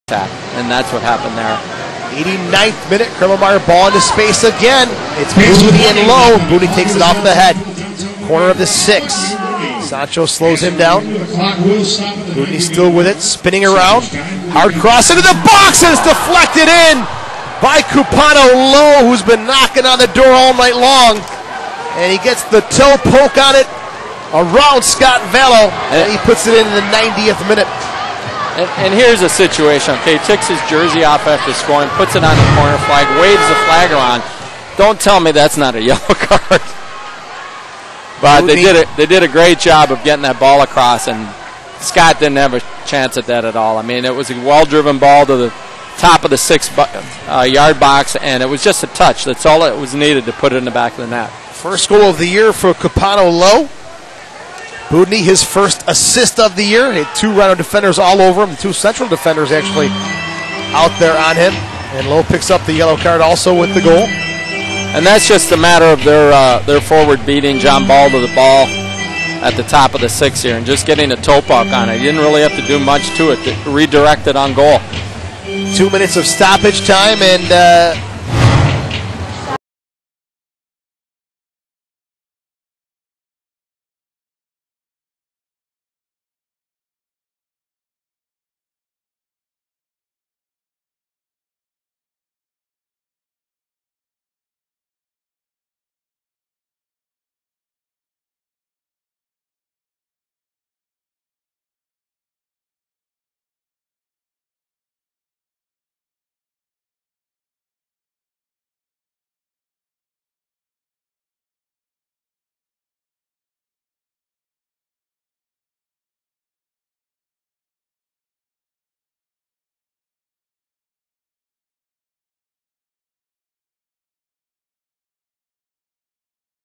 Kupono Low scores against the Rochester Rhinos
Kupono Low scores his first goal of the season against the Rochester Rhinos on August 5, 2009.  After the goal, Low had one of the best celebrations ever.